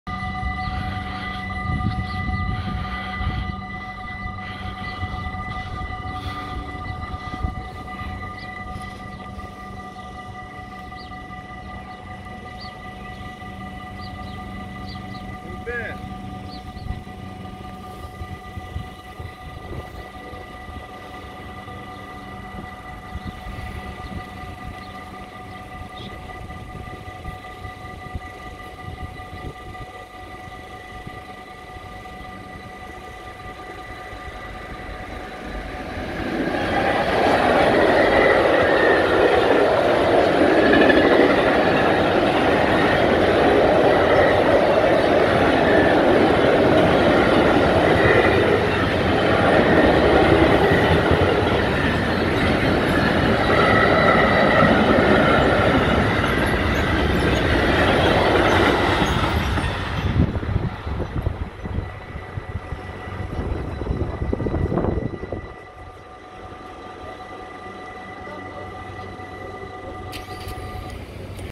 17 mei 2019
Hier passeert de DB Cargo 189 met Lege DB Autotrein Van Amersfoort via Apeldoorn,Deventer,Almelo,Hengelo,Bad Bentheim(D),Osnabrück met als Eindbestemming Wolfsburg(D).

Amersfoort,Hogeweg